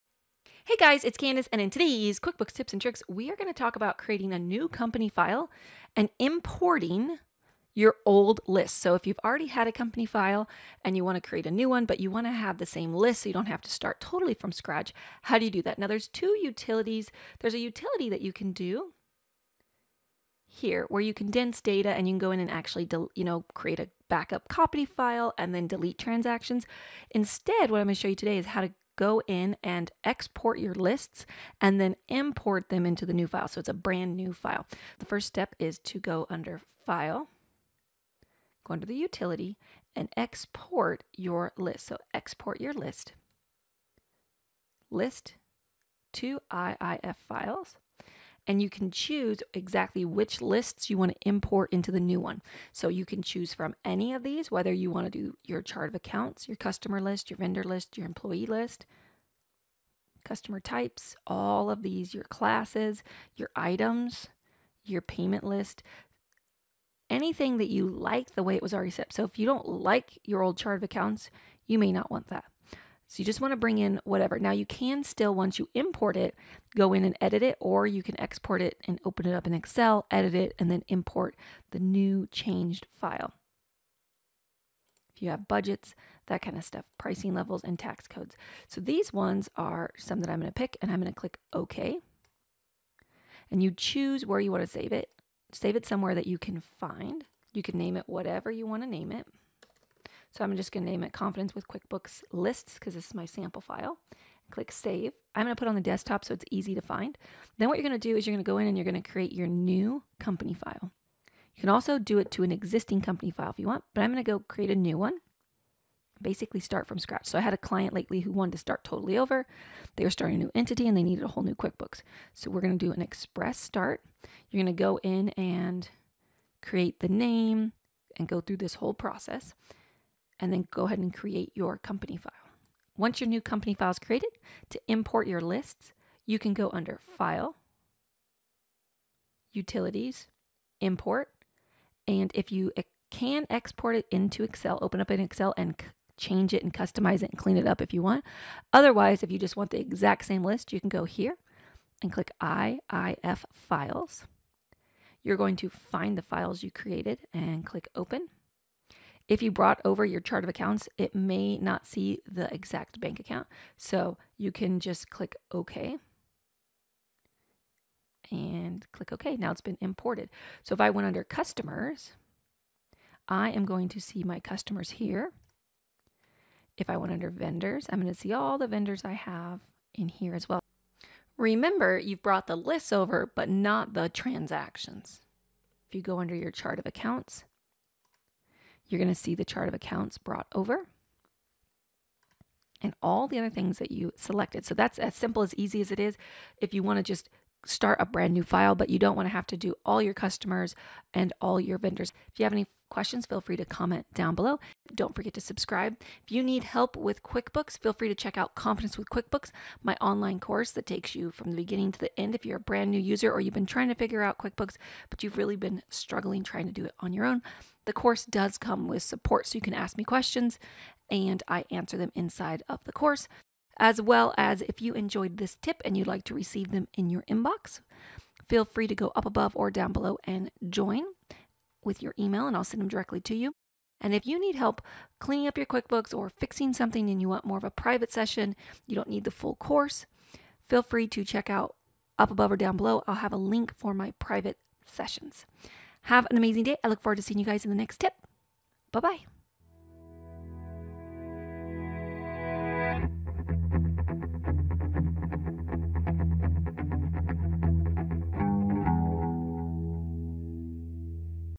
Create a new company file and import lists from an existing QuickBooks company
Learn how to Import list data from an existing QuickBooks company…

Are you creating a new company file in QuickBooks?

One way to save time is to import your lists from a previous QuickBooks file or Excel list.

Step 1. Make sure you back up your data file before you start 🙂

Step 2. Export your list  (File, Utilities, Export, Lists to IIF Files…)

Step 3. In the new company file Import the lists. (File, Utilities, Import, IFF File)

One way to save time if you have a previous QuickBooks file is to export the lists (customers, vendors, chart of accounts...) and import them into the new file. You will get the list detail but no transactions.

➡️ Exporting List

Go up to “File”
“Utilities”
“Export”
“List to IIF Files…”
Select the list you want to export
Click “OK”
Choose where you want to save it
“File name”
Click “Save”
Click “OK”

**You can create a new company file or existing company file.

“New Company File” or “New Company from Existing Company File…”
“Express Start”
Add your details

➡️ Importing List

“Utilities”
“Import”
“Excel Files..”/ ”IIF Files..”
Find the file you want to import
Click “Open”
Click “OK”

➡️ Remember: You brought the list over but not the transactions.

Timestamps:

0:00 - Intro
0:15 - Condense Data
0:28 - Export List in QuickBooks
1:47 - Create new company file
2:14 - Import list in QuickBooks
3:00 - Remember 💡
3:05 - Chart of Accounts
3:22 - Final Thoughts